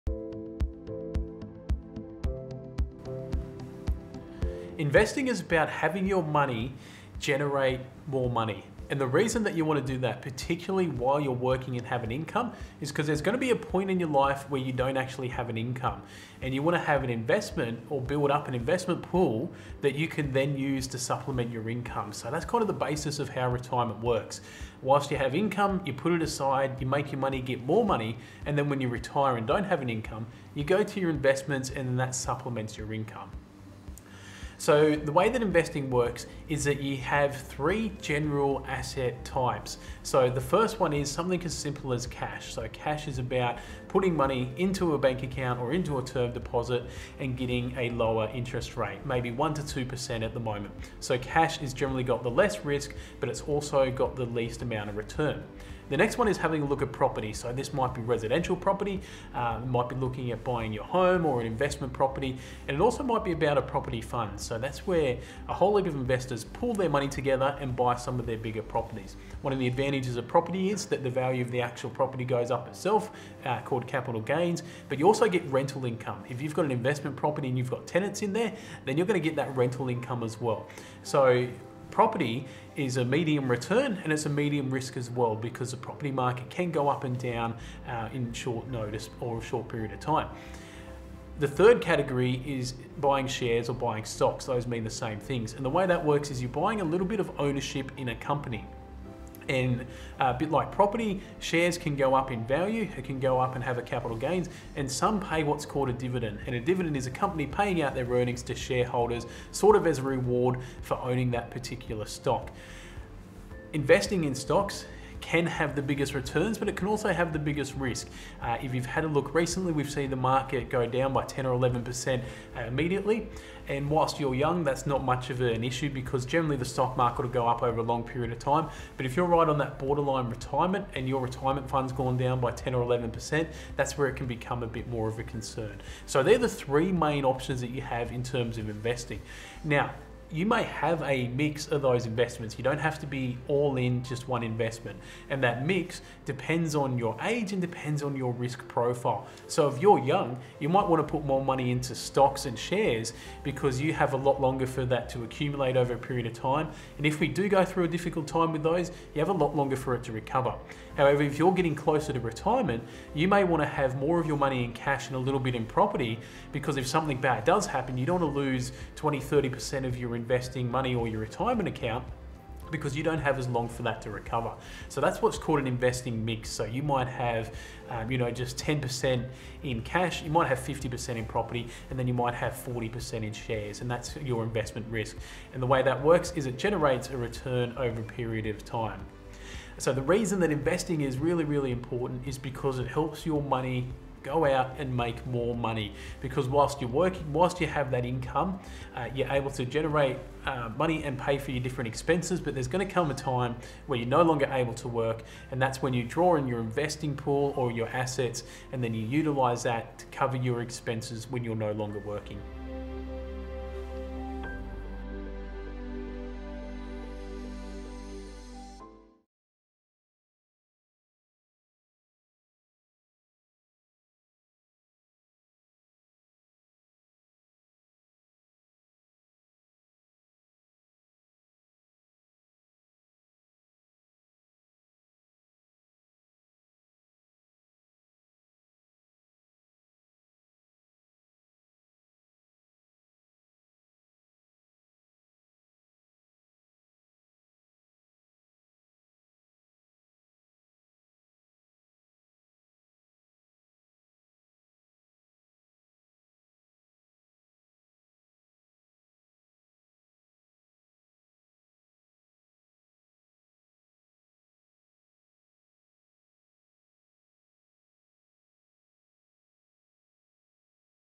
Investing
Foundations of Finance.

Phil discusses some principles of investing.